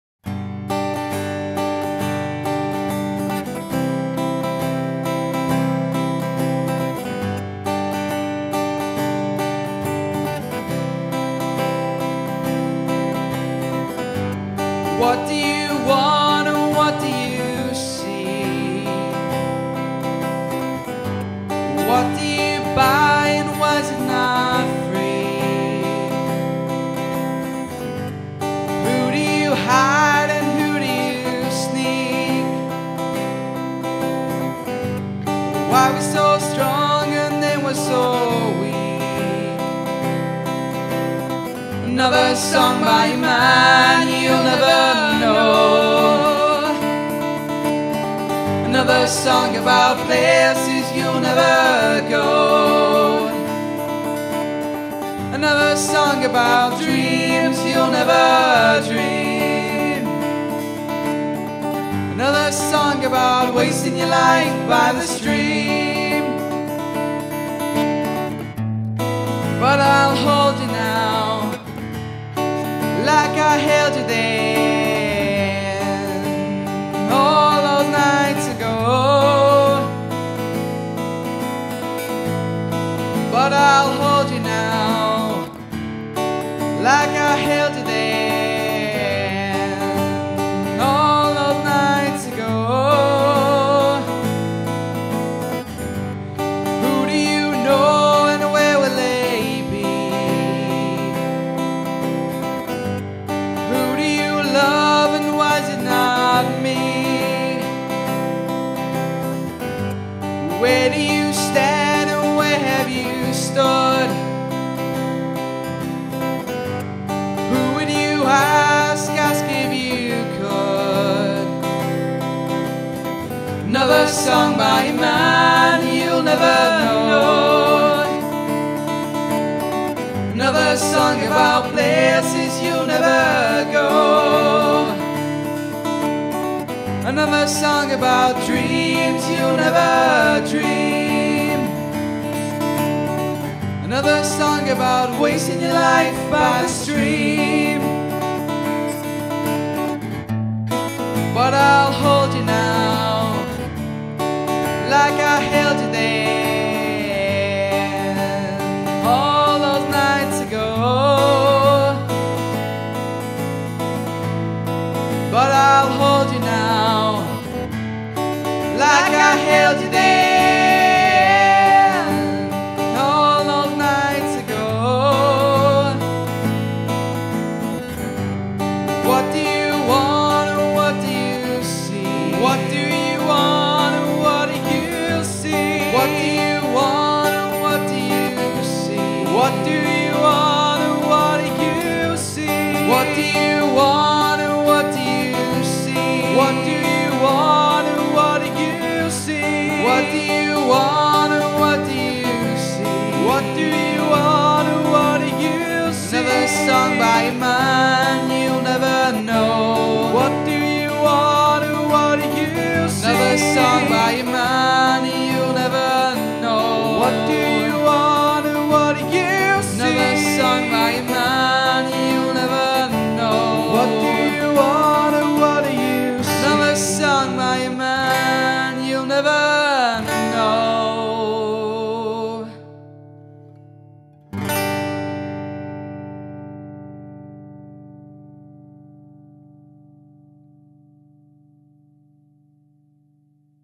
We Steal Flyers - Another Song Cover
We Steal Flyers are taking part in a 100 day tour to raise money for Grace House North East children's hospice appeal. I hope you like this cover.